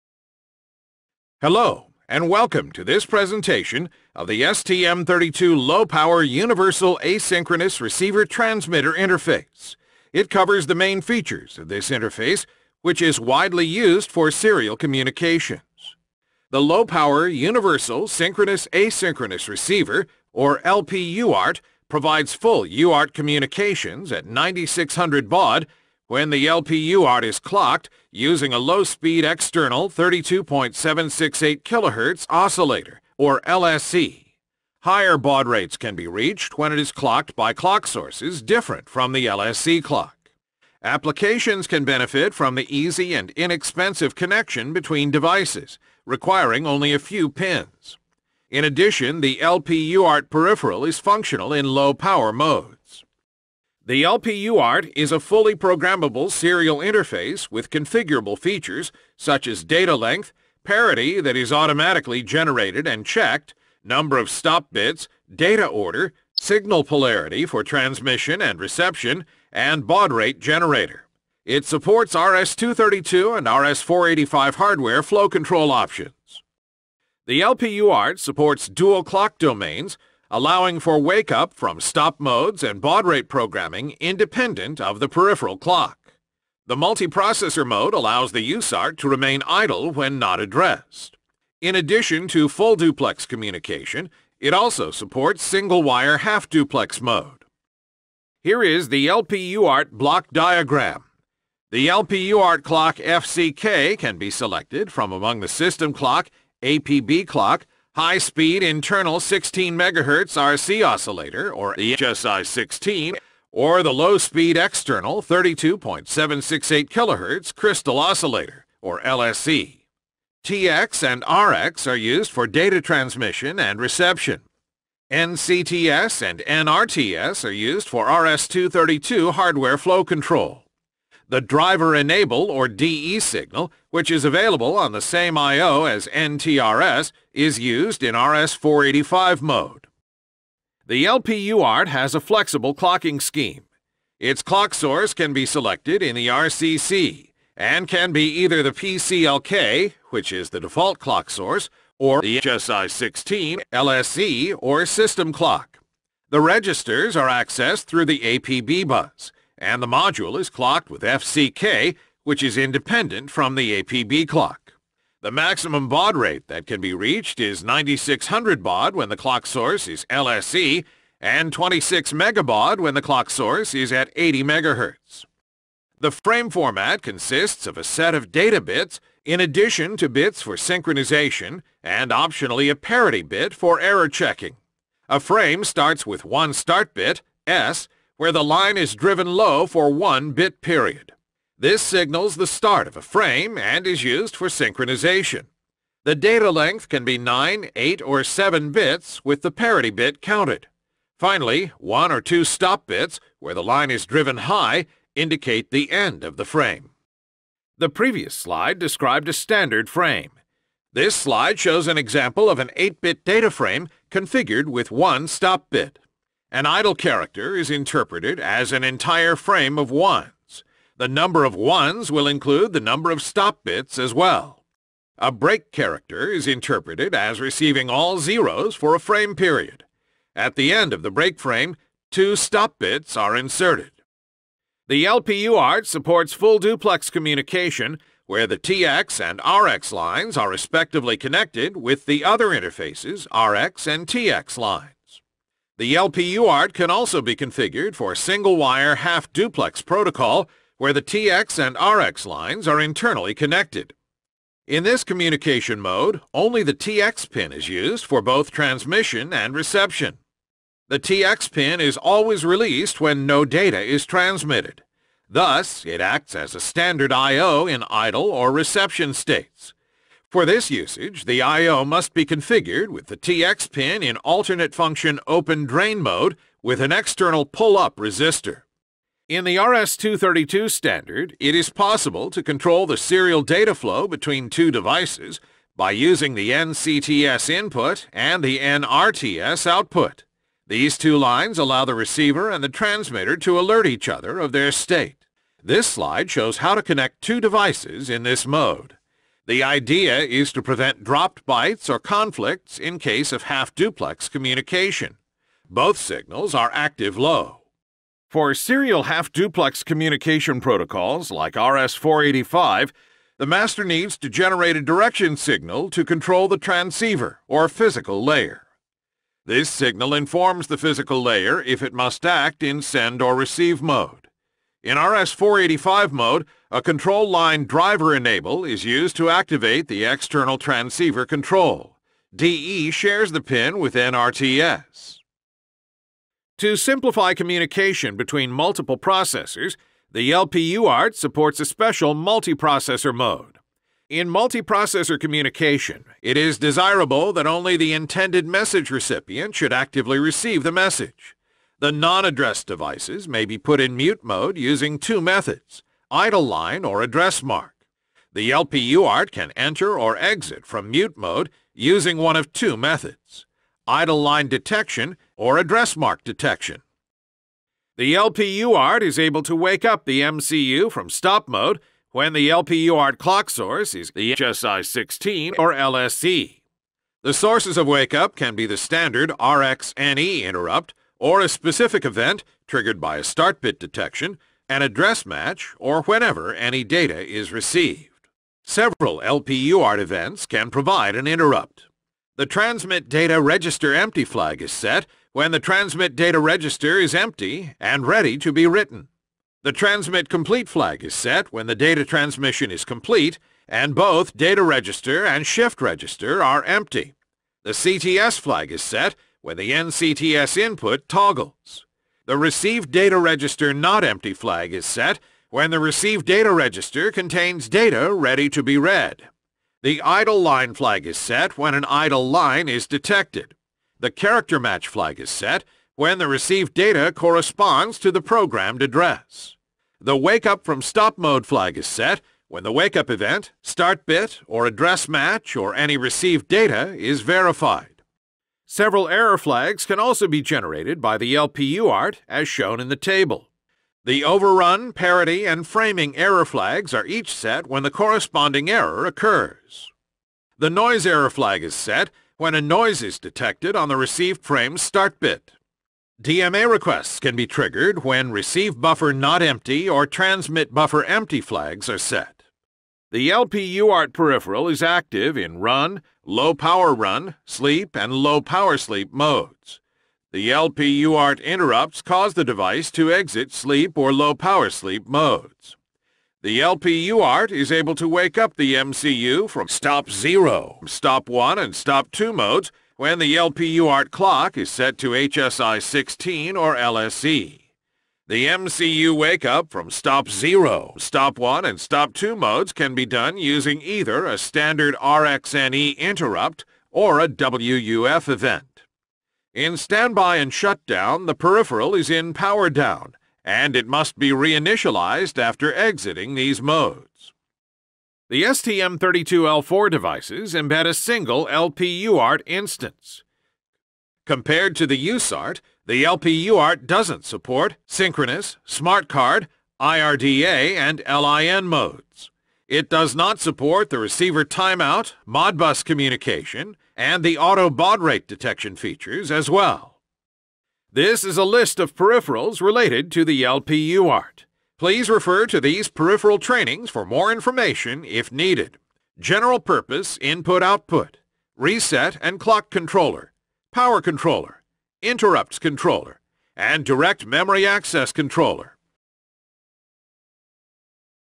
STM32L4 OLT - Low-power universal asynchronous receiver transmitter (LPUART) [한글자막]
STM32L4에 내장된 Low-power universal asynchronous receiver transmitter (LPUART)에 대해 설명합니다.
Low power모드에서도 동작할 수 있게 설계된  LPUART장치의 주요 특징에 대해 다룹니다.

아래 사이트들에서 더 많은 자료를 확인하세요.